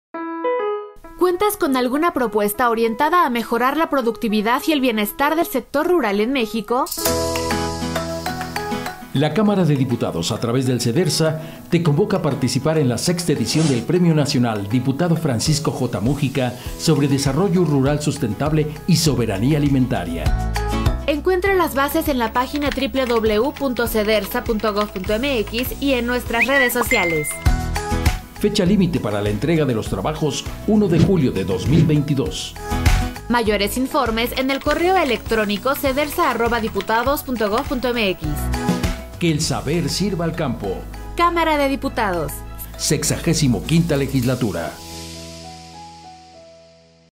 Sexta Edición del Premio Nacional Dip. Francisco J. Múgica
La Cámara de Diputados, a través del CEDRSSA, te invita a participar en la Sexta Edición del Premio Nacional Dip. Francisco J. Múgica, sobre productividad y bienestar del sector rural en México.